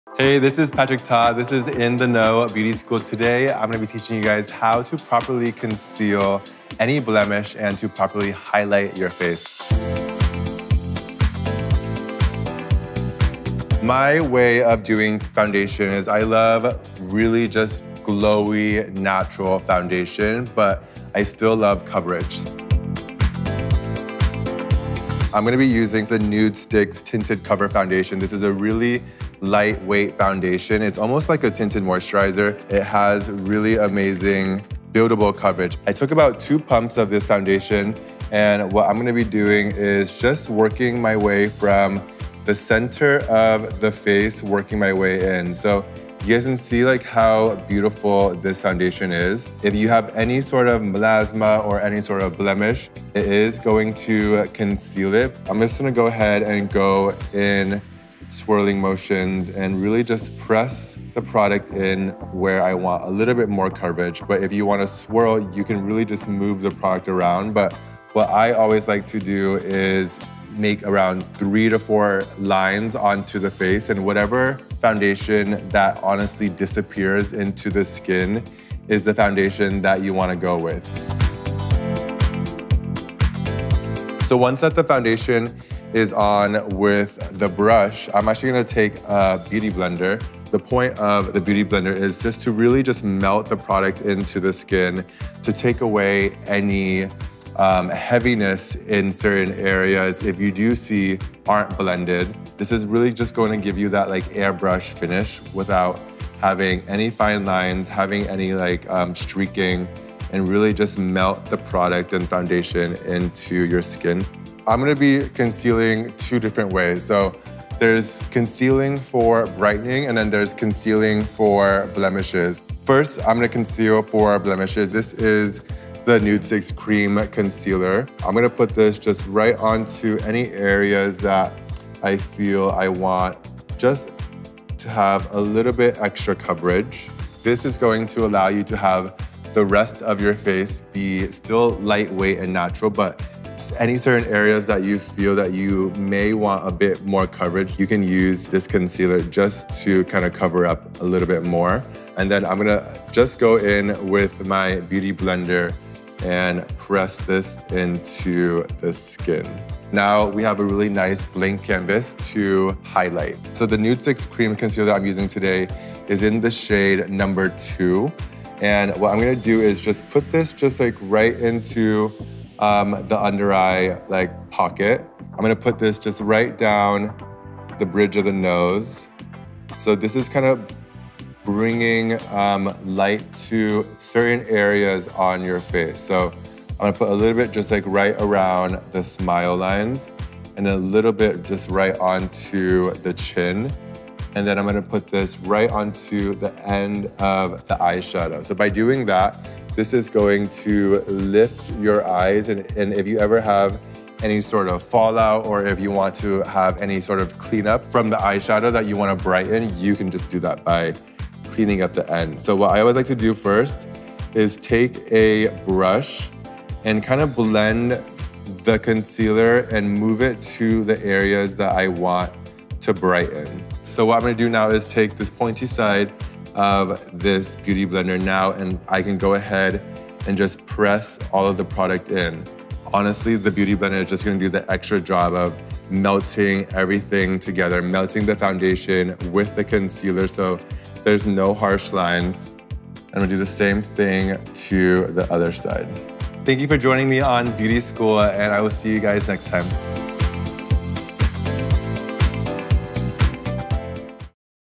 Conceal and highlight like a pro with this tutorial
Patrick Ta shows us how he conceals and highlights for a flawless finish. Shop his favorite products below and read the full article

Our team is dedicated to finding and telling you more about the products and deals we love. Pricing and availability are subject to change.